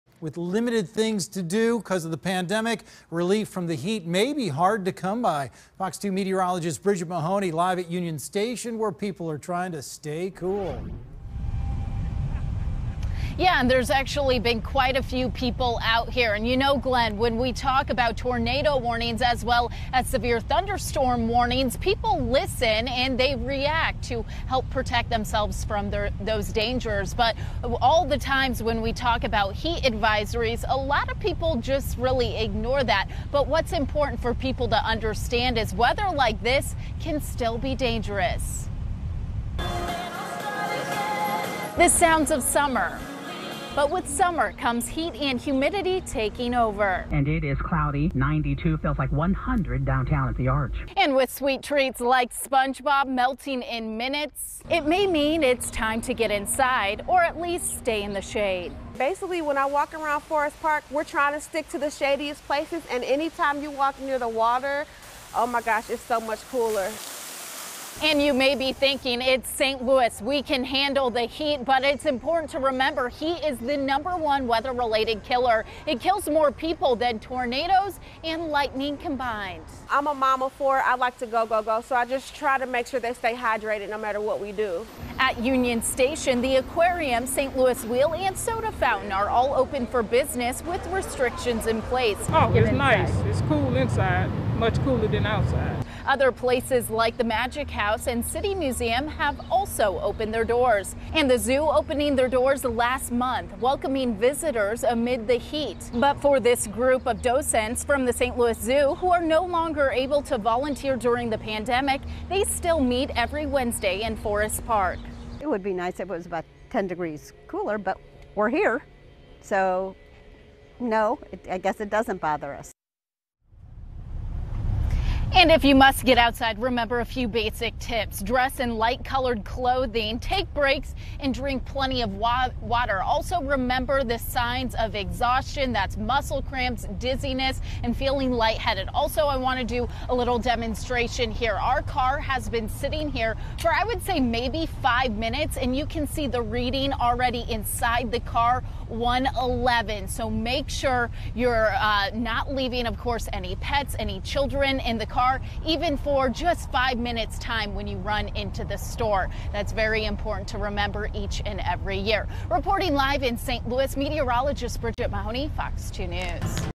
Relief from the heat may be hard to come by during pandemic
With a heatwave bringing heat indices up to the triple digits for consecutive days, finding a break from the heat can be hard for some in times of social distancing.